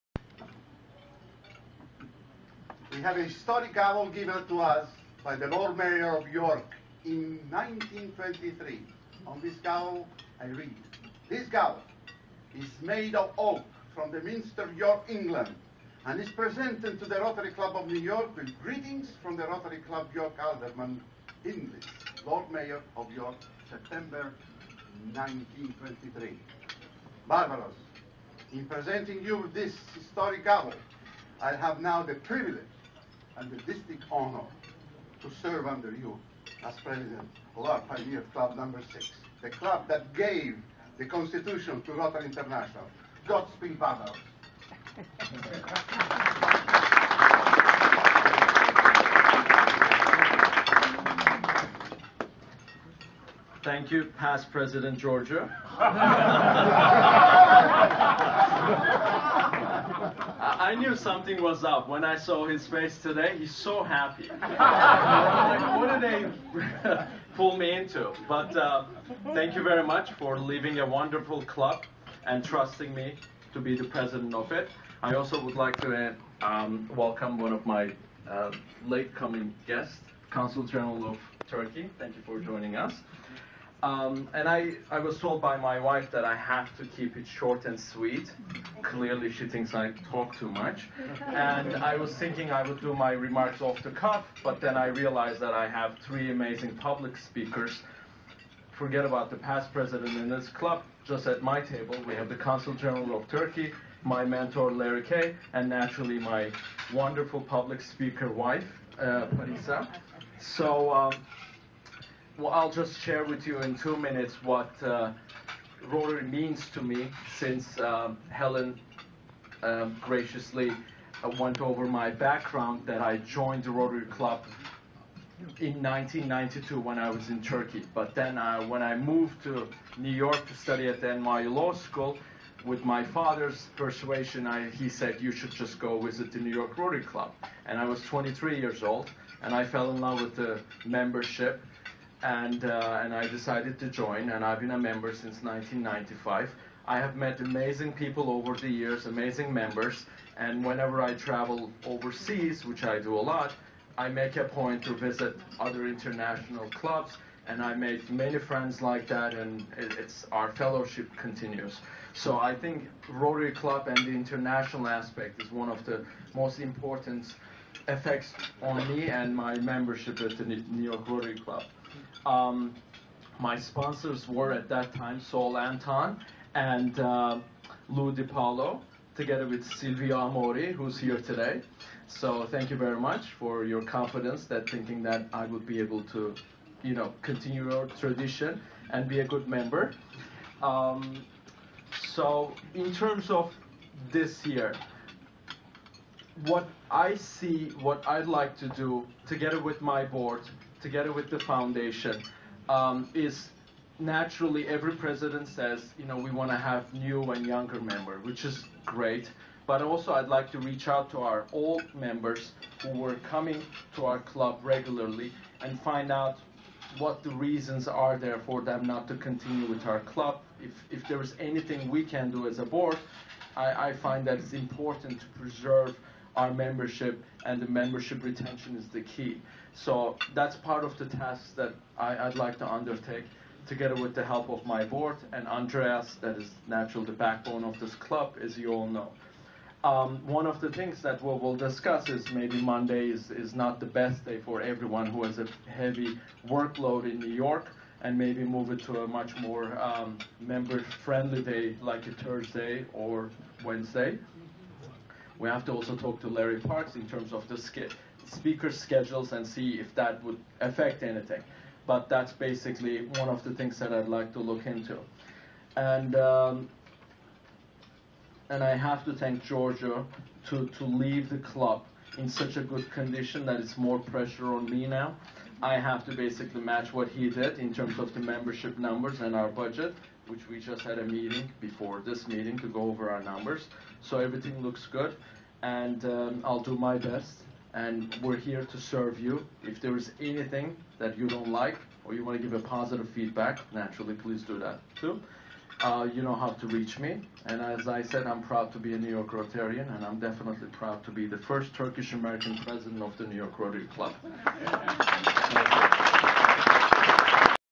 103 Yıllık New York Rotary Kulüp İlk Türk Başkanı Barbaros Karaahmetoğlu Oldu
ABD'nin en eski Rotary klüplerinden biri olan New York Rotary Club 103 yıllık tarihinde ilk kez bir Türkü başkan olarak seçti. Avukat Barbaros Karaahmetoğlu, dün yapılan törenle New York Rotary Kulübü başkanlığını Giorgio Balestrieri'den devraldı. 25 Haziran 2012